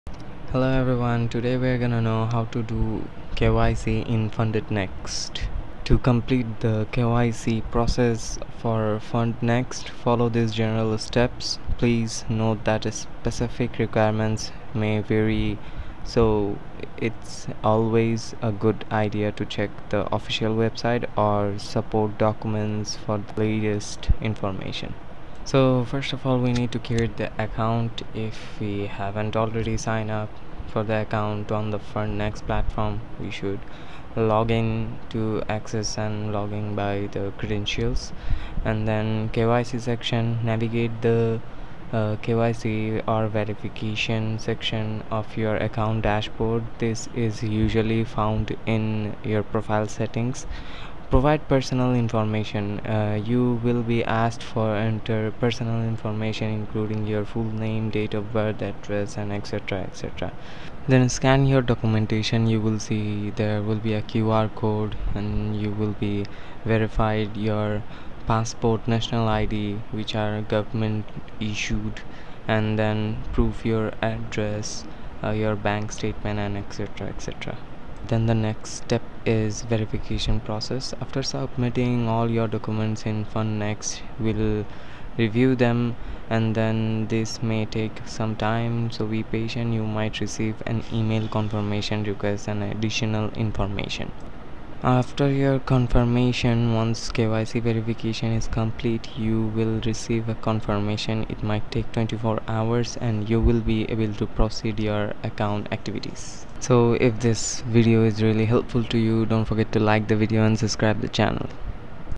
How To Do KYC In FundedNext And Submit Documents Properly For Verfication
How To Do KYC In FundedNext – Need to complete KYC verification in FundedNext? In this video, we’ll guide you through the KYC process, from submitting your ID and documentation to finalizing your account verification.